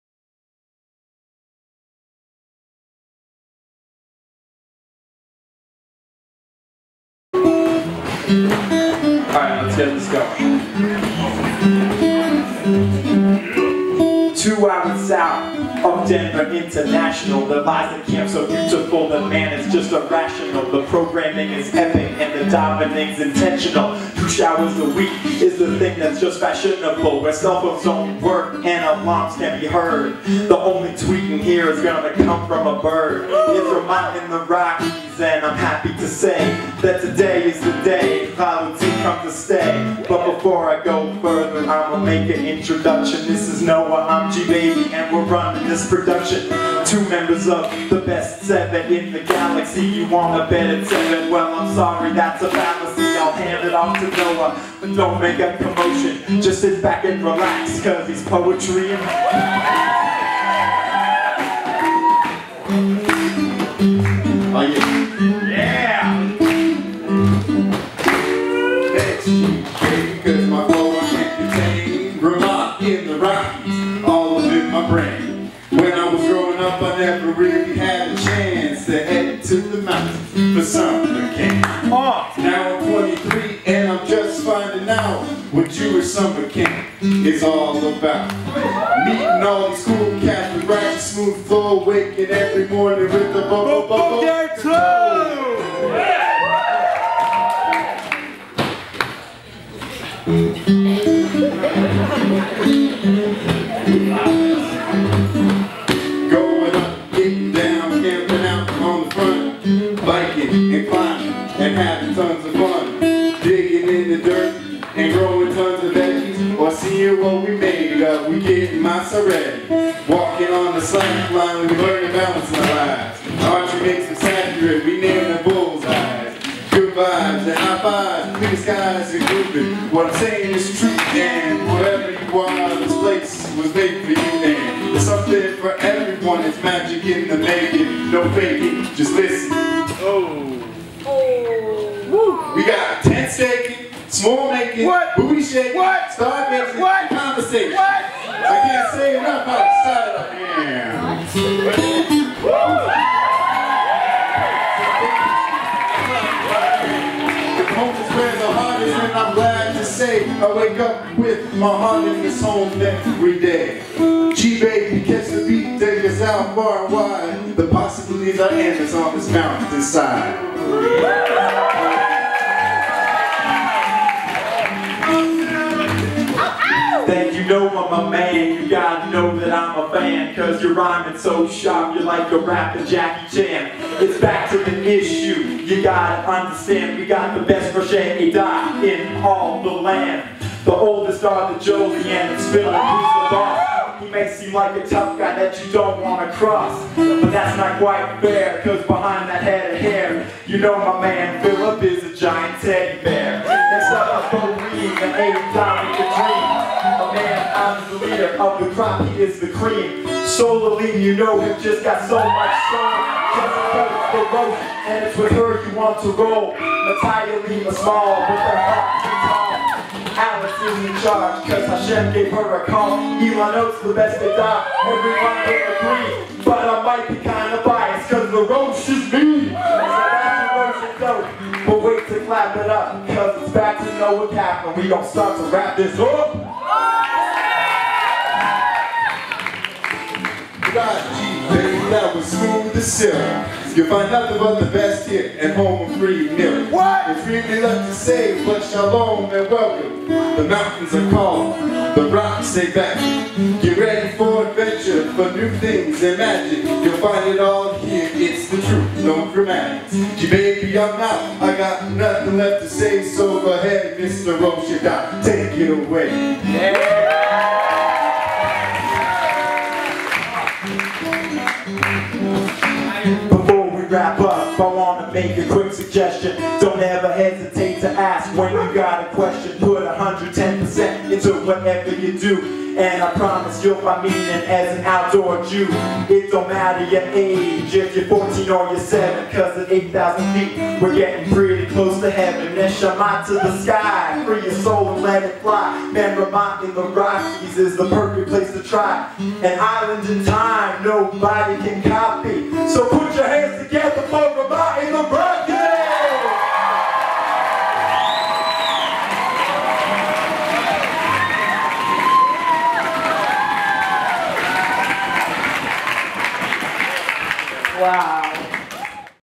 Welcome to Ramah in the Rockies
Original Rap by Gabi Wasserman and Noah Kaplan